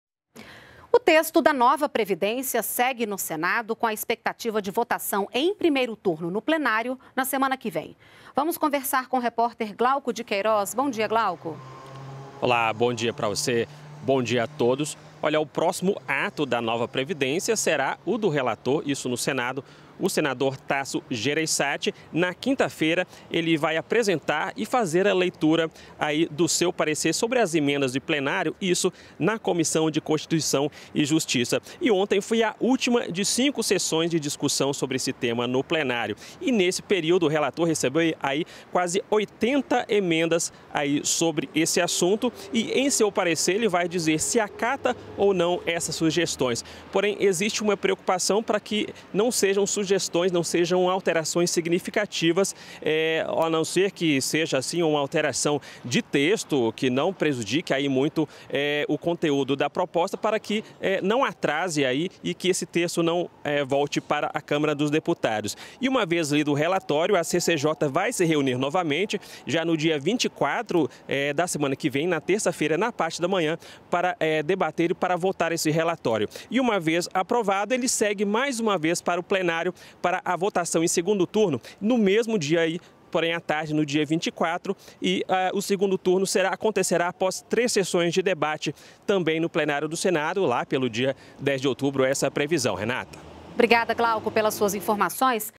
Nova Previdência segue no Senado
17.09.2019 - O texto da Nova Previdência está sendo debatido no Senado Federal e a expectativa é de que seja votado na semana que vem.